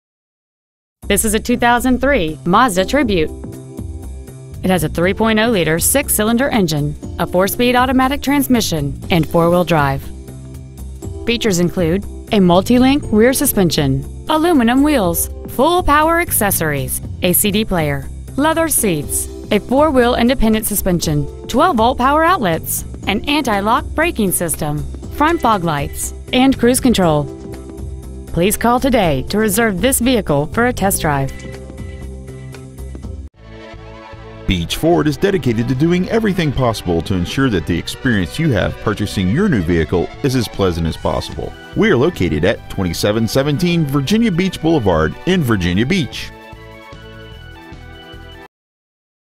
Used 2003 MAZDA TRIBUTE Virginia Beach VA
We are proud to present this 2003 MAZDA TRIBUTE .

 Year : 2003

 Make : MAZDA

 Model : TRIBUTE

 Engine : 3.0L V6 24V MPFI DOHC

 Trans . : 4-SPEED AUTOMATIC

 Exterior : DARK TITANIUM METALLIC

 Miles : 97,113

 2717 Virginia Beach Blvd